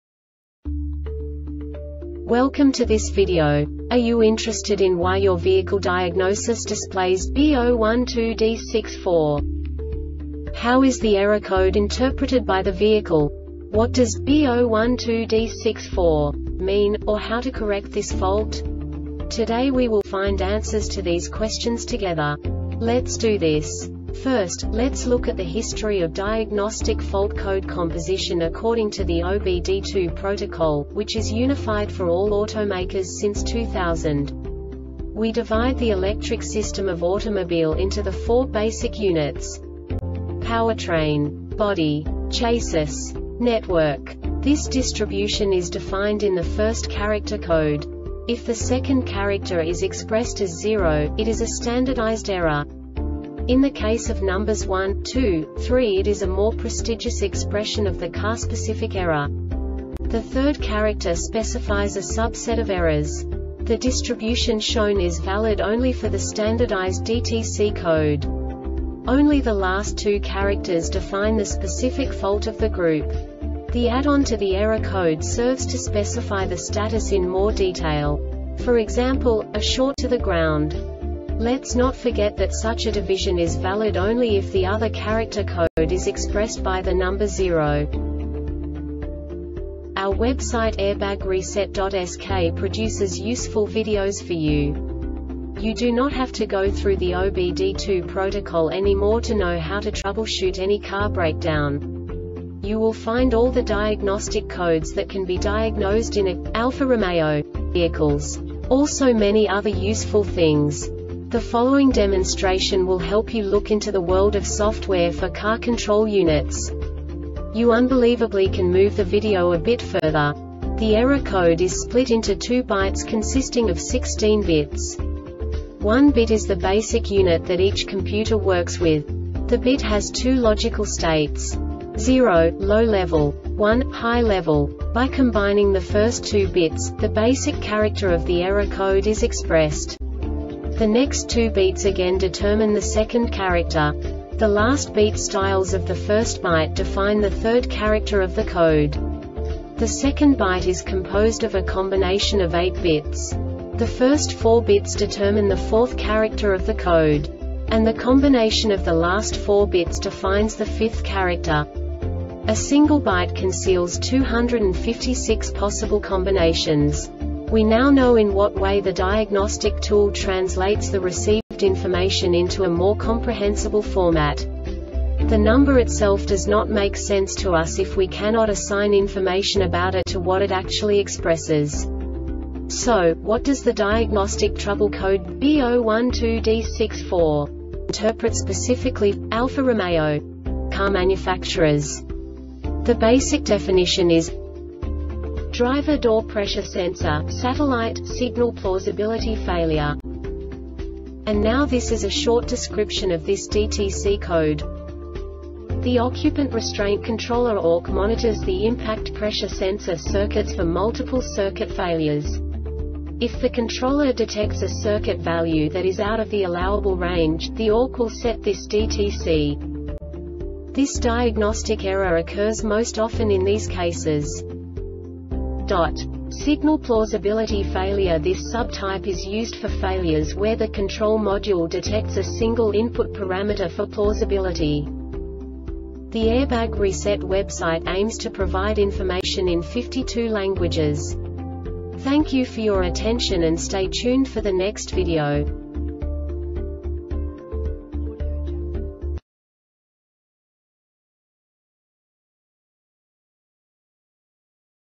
DTC Alfa-Romeo B012D-64 Short Explanation
Contents:
0:21 Basic DTC analysis according to OBD2 protocol standard.
1:48 Insight into programming
2:58 A diagnostically understandable description of the Diagnostic Trouble Code
3:05 Basic error definition

Make:
Alfa Romeo
Code:
B012D-64
Definition:
Driver Door Pressure Sensor (Satellite)-Signal Plausibility Failure
Description:
The Occupant Restraint Controller (ORC) monitors the Impact or Pressure Sensor circuits for multiple circuit failures. If the controller detects a circuit value that is out of the allowable range, the ORC will set this DTC.